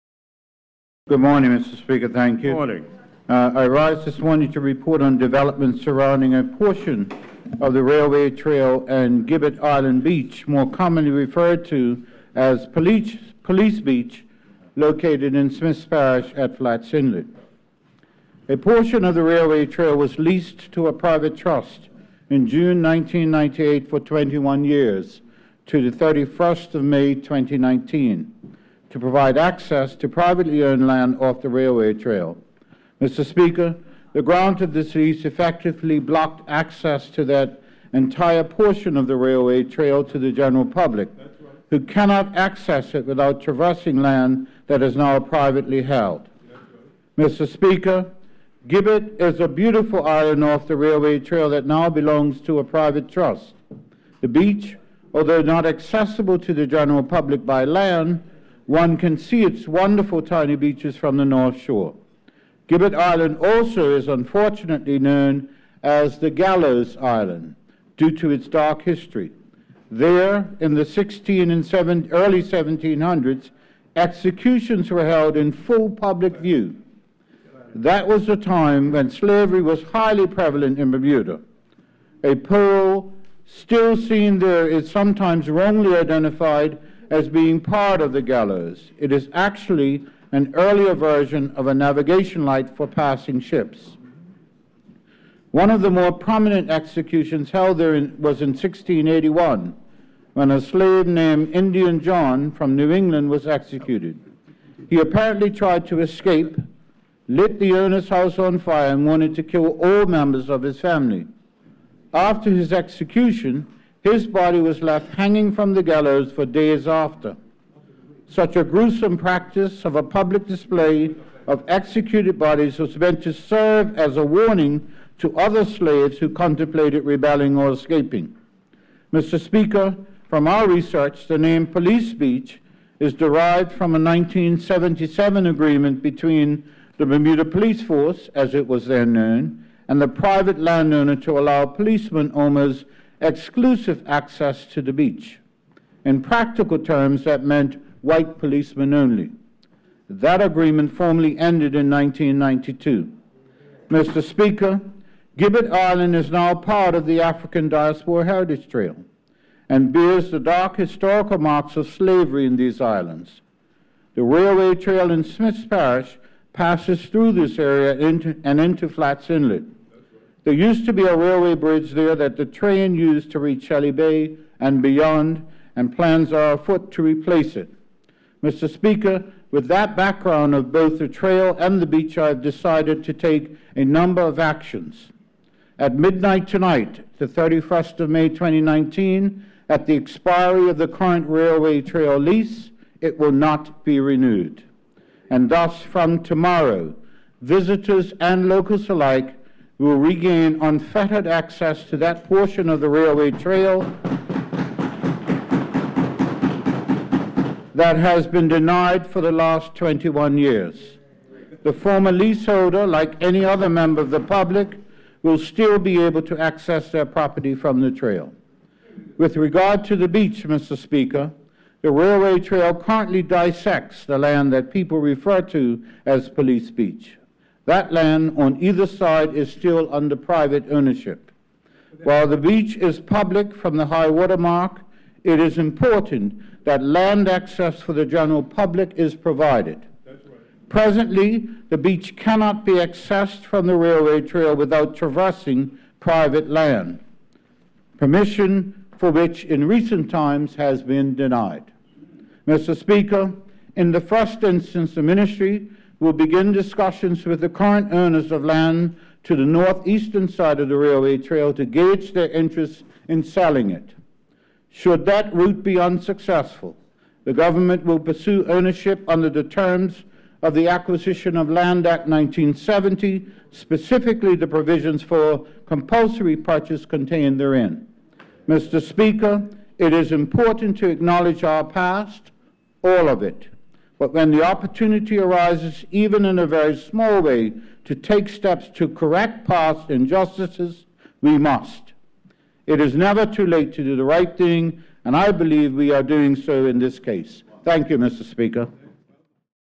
Railway Trail and Gibbets Island
The Hon. Lt. Col. David Burch, JP, MP, Minister of Public Works delivers a Ministerial Statement to the House of Assembly on Railway Trail and Gibbets Island on Friday, May 31, 2019.